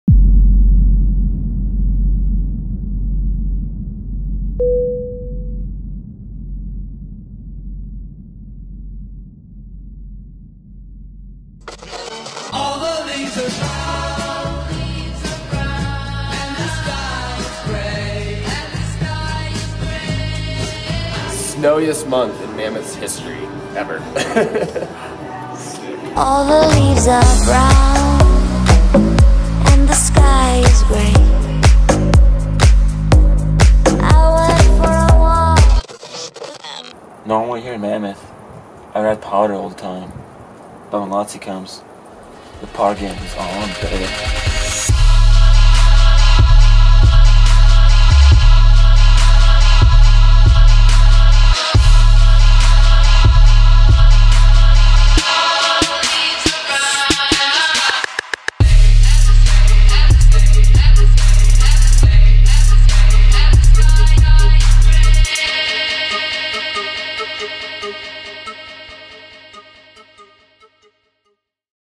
MakeAmaricaSickAgain Ep.1
First video drop from Cali! Thank you Mammoth Mountain for once-in-a-lifetime snowpark and Branko Konge for hosting me :) Cant wait to shred more!

Bataleon snowboards,  HORSEFEATHERS CLOTHING, BIG SHOCK, Causabau, Zabil.cz/sk